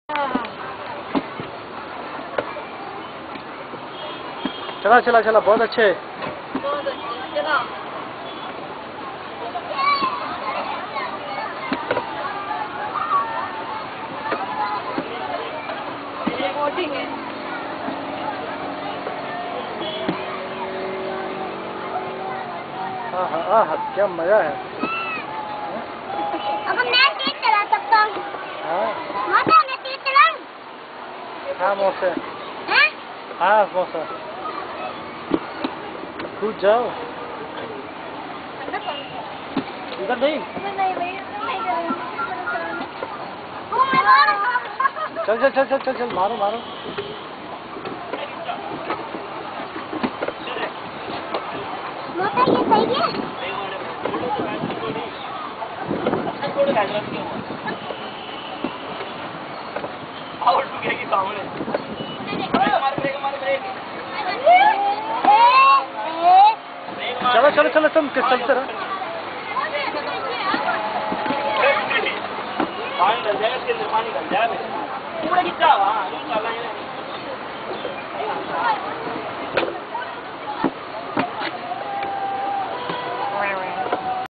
Preeti & suchi enjoying boating at Purana Qila
Preeti,kavyansh & suchi enjoying boating at Purana Qila new delhi.
Full masti and enjoyment